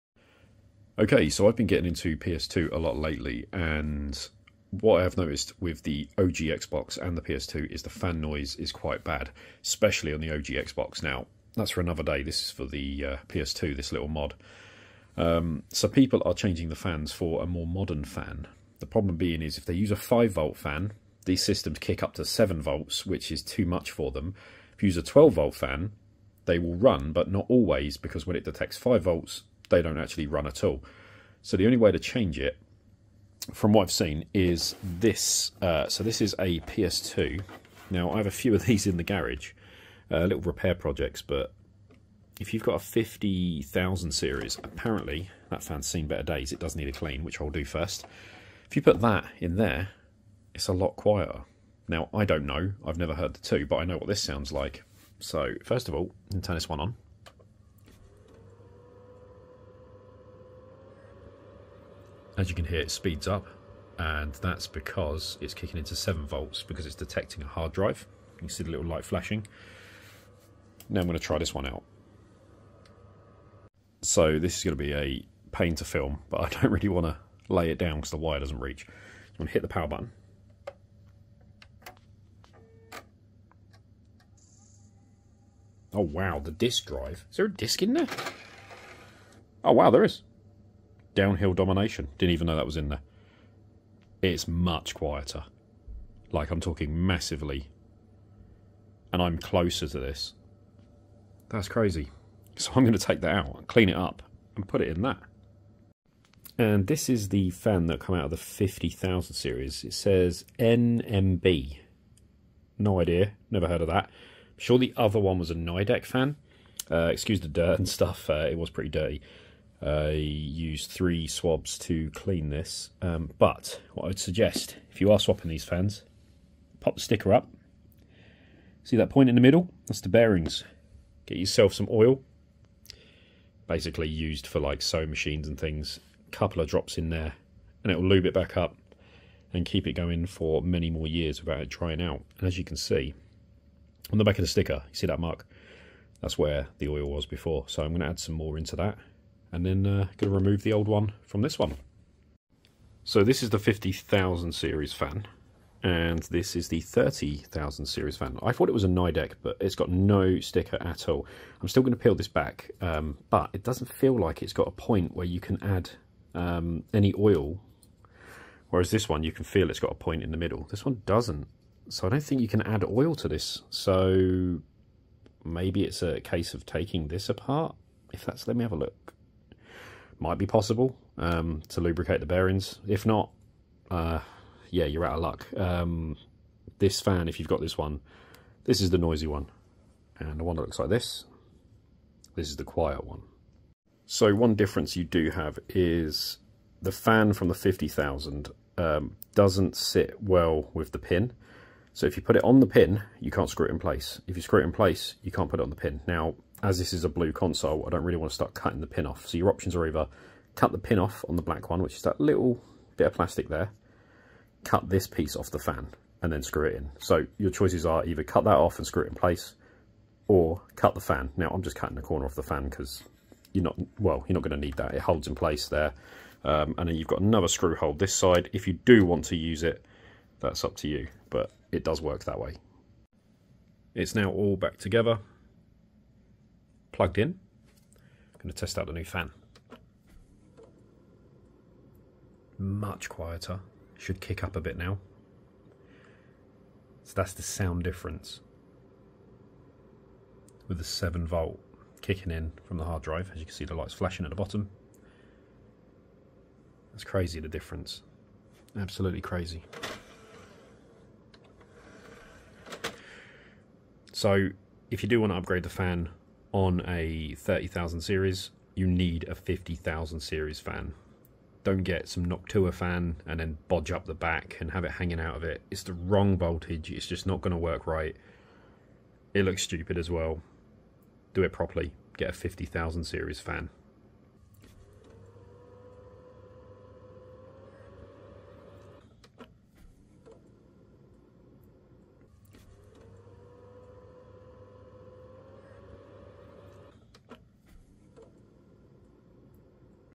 Upgrading the Sony PS2 Fan the PROPER way with a 50k fan
if you have a 30k series and want a quieter time
Get the fan from a 50k series
cut off the two pin tabs of plastic
screw in place as normal after swap
game on!

the modern 12v fans dont spin enough due to lack of power producing more heat and the 5v fans speed up to 7v which is not the voltage they are made for so use the 50k fan instead its worth it!